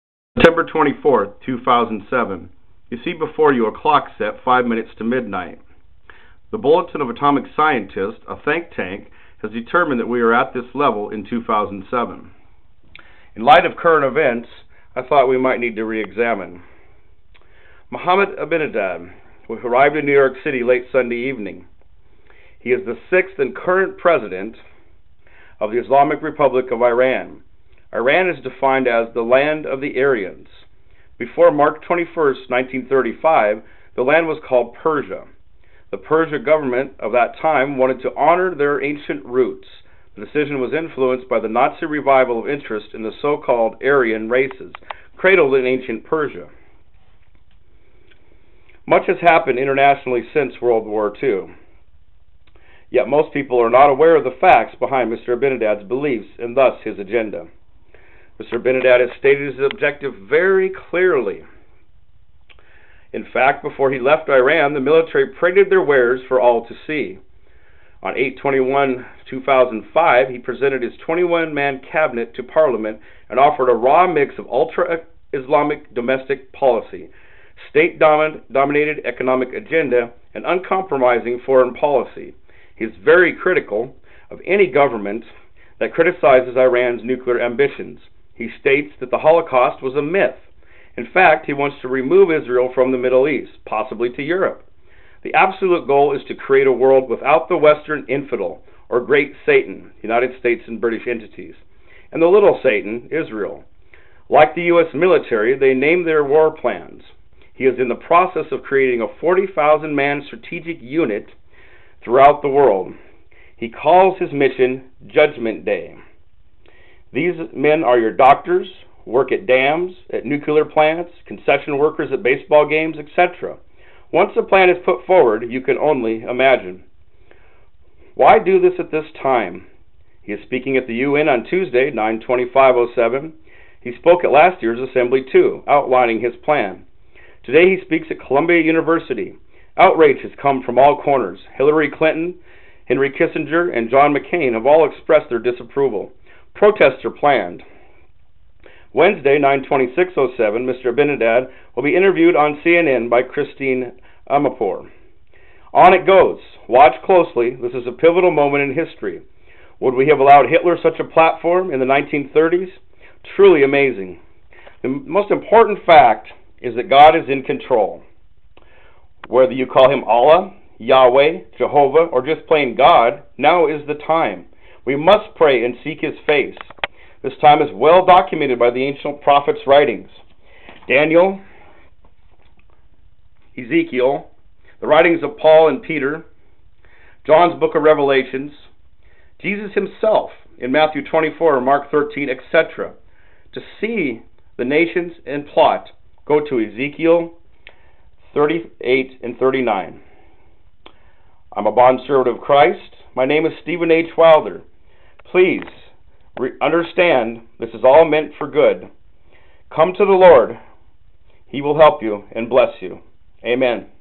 The coming of the 12th Imam~~AHMADINEJAD
AHMADINEJAD spoke at the at United Nations today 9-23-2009. This video was made in response to his appearance in 2007.Same tired speech ~death to Israel, the Holocaust was a myth (LIE!!) etc  The big difference is Iran is months away from producing a Nuclear Weapon!! AHMADINEJAD says they are their military is 10 times stronger then before!! The stage is set!!

This video is information, nothing is teaching.
2)Iran/Persia is just basic history. It can be found in any good encyclopedia.
3)The Bible. Ok here is where it is up to faith. That's fine...The objective was to provide hope. Nothing I said is any different then Billy Graham or Hal Lindsey (Joel Rosenberg etc).

Mahmoud Ahmadinejad the President of Iran spoke at Columbia University in upper Manhattan,New York City USA.  9/25/07 he addresses the UN general Assembly. He is very well versed and pointed in his message. This video is a response...

Original Release on Youtube channel 'Yakwild" (closed)...